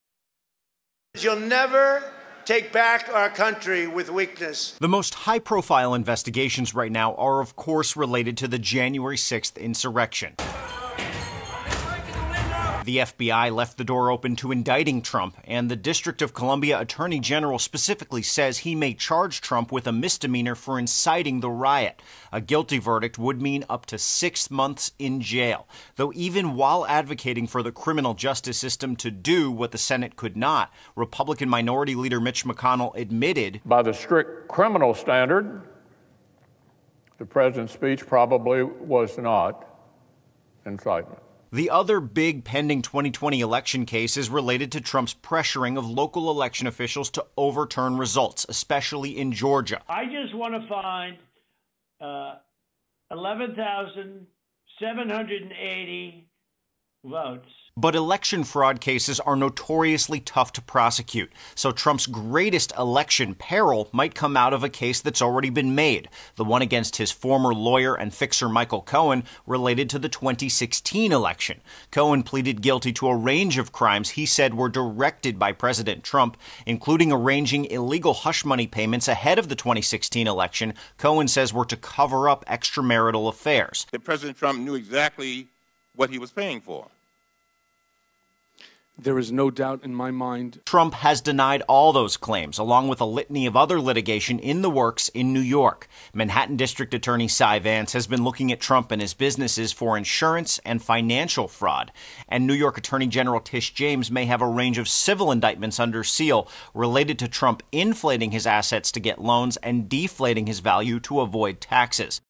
Now a private citizen, Trump facing legal woes
Manhattan District Attorney Cyrus R. Vance Jr., is in the midst of an 18-month criminal grand jury investigation focusing in part on hush-money payments paid to women on Trump’s behalf, and whether Trump or his businesses manipulated the value of assets — inflating them in some cases and minimizing them in others — to gain favorable loan terms and tax benefits.